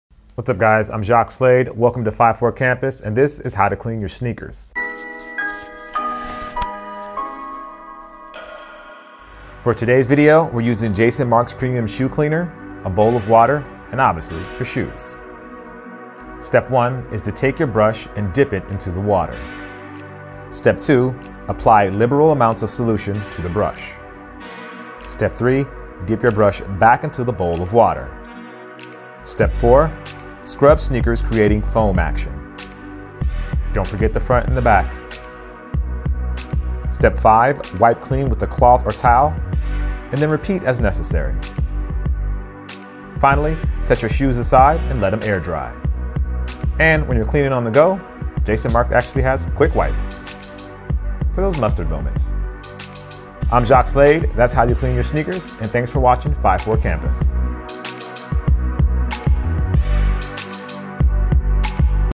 5 Easy Steps to Clean Sneakers with Jacques Slade
Sneaker expert Jacques "Kustoo" Slade demonstrates the easiest way to clean your dirty kicks using Jason Markk Premium Shoe Cleaner.